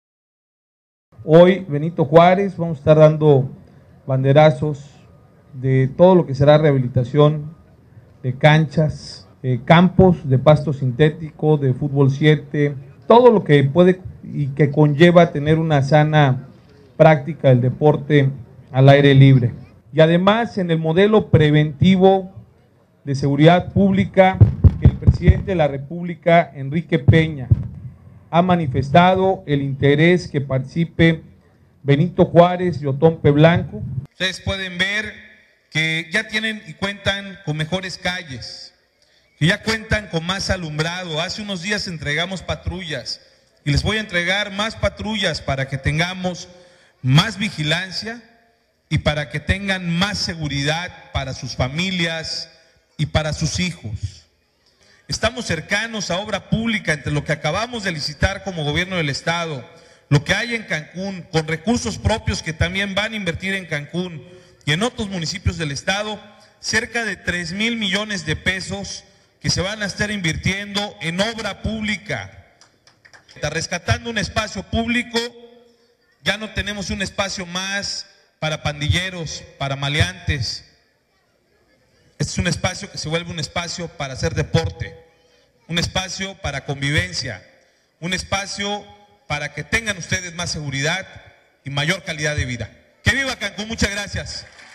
02DIC13 BANDERAZO DE INICIO DE OBRAS DE CANCHAS DE FÚTBOL Y DE USOS MÚLTIPLES EN CANCÚN
* Se construye una cancha de Fútbol 7 y de Usos Múltiples, en la Supermanzana 21; y se rehabilita una cancha de fútbol en la Región 230, cuya inversión global asciende a casi 8 millones 500 mil pesos
* El jefe del Ejecutivo asegura que continuará con obras que promuevan el deporte entre la ciudadanía y complementen el modelo preventivo en materia de seguridad pública, que impulsa el presidente Enrique Peña Nieto
* Paul Carrillo de Cáceres, presidente municipal de Benito Juárez, reconoció la labor y la gestión de recursos del Gobernador ante la Conade, para llevar al cabo estas obras de gran importancia para la sociedad cancunense
* Francisco Pool Mukul, vecino de la Región 230, de manera muy efusiva, dijo que desde hace 10 años anhelaba tener en su colonia una cancha digna para jugar fútbol, "y hoy gracias al Gobernador, mi sueño se hará realidad"

CANCÚN, Quintana Roo, 2 de diciembre.— El gobernador Roberto Borge Angulo dio el banderazo de inicio para las obras de construcción de la Cancha de Fútbol 7 y de Usos Múltiples de la Supermanzana 21, y la rehabilitación de la cancha de fútbol del mismo tipo en la Región 230 de esta ciudad, cuya inversión global es de casi 8 millones 500 mil pesos.